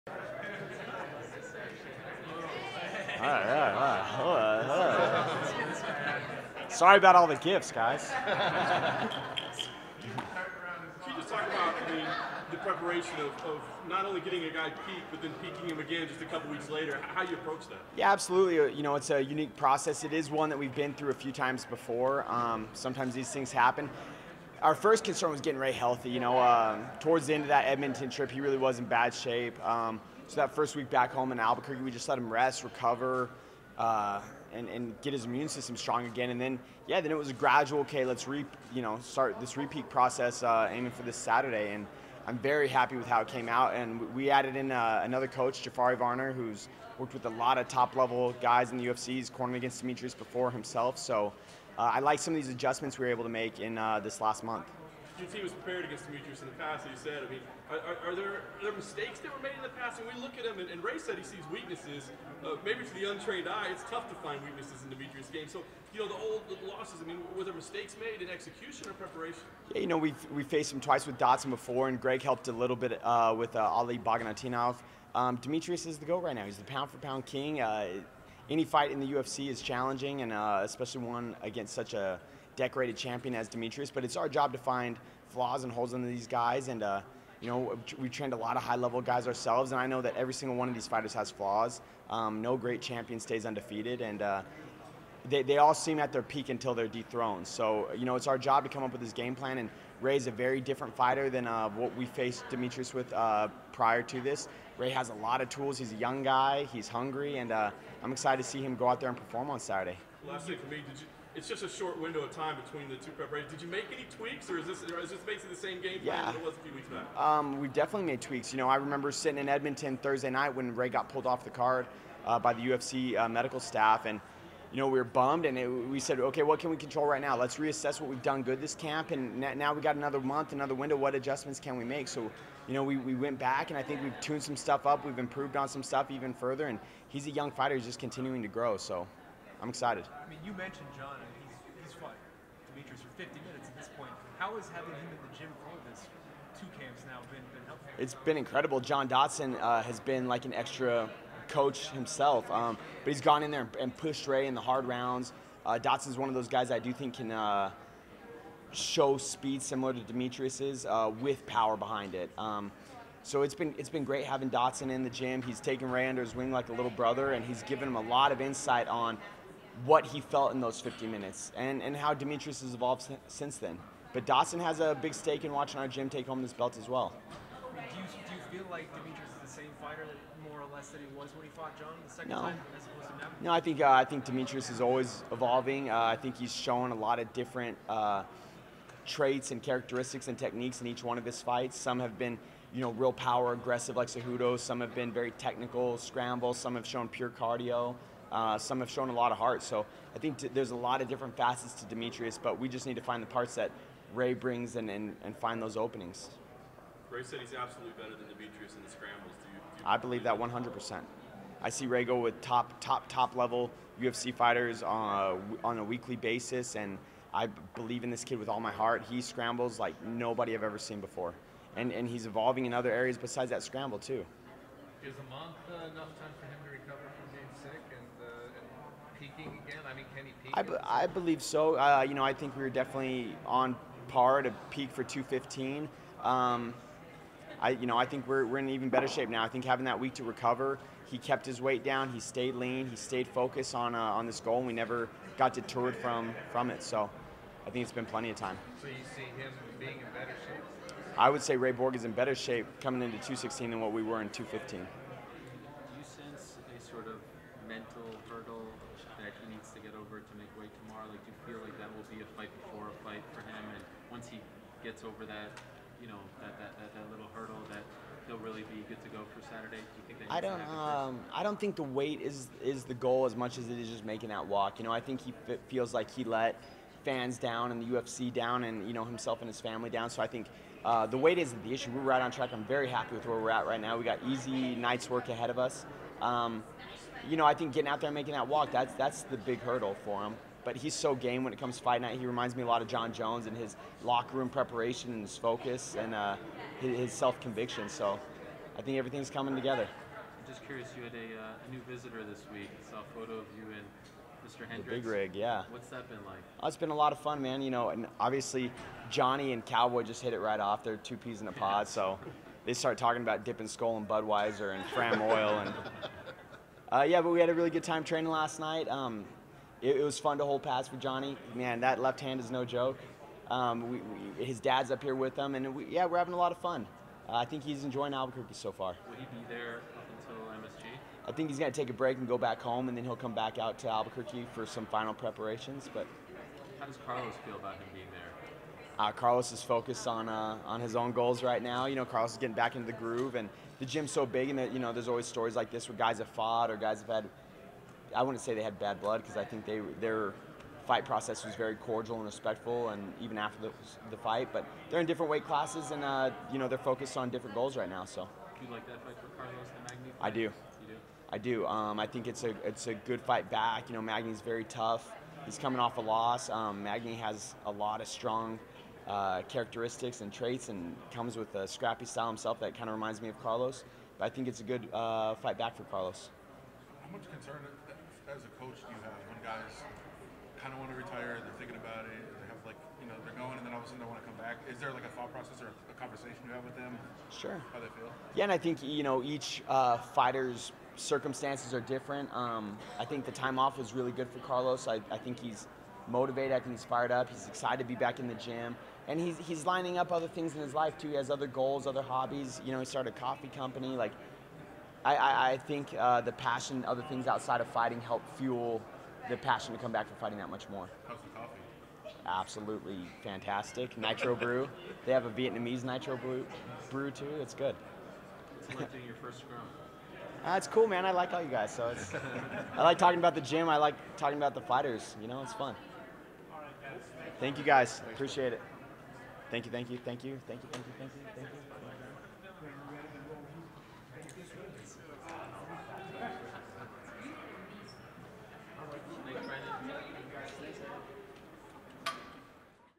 Brandon Gibson Talks Ray Borg, Johny Hendricks and Carlos Condit - MMA Fighting
Ray Borg coach Brandon Gibson discusses his fighter's preparations for UFC 216, Johny Hendricks joining their team, and Carlos Condit's return during a pre-fight scrum Thursday.